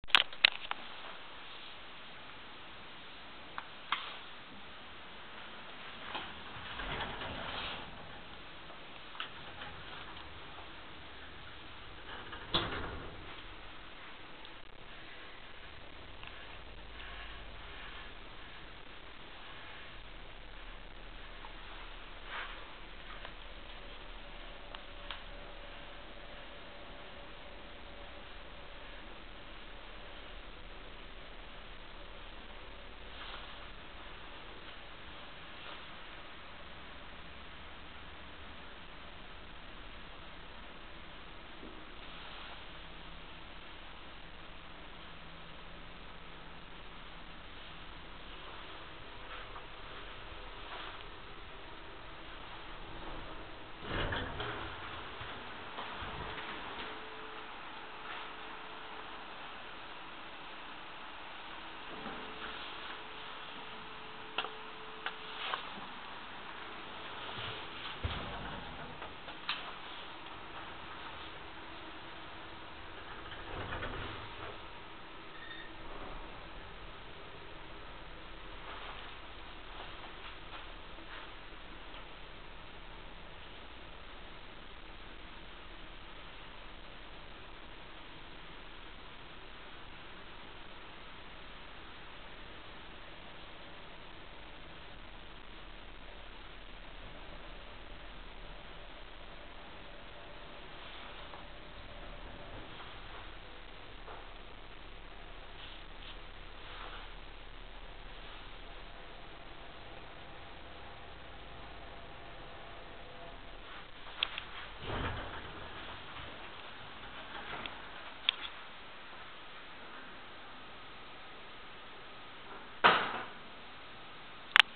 KONE Elevator in Chinese restaurant of Raahe
Going up and down with KONE Elevator at Chinese Restaurant Hai Long in Raahe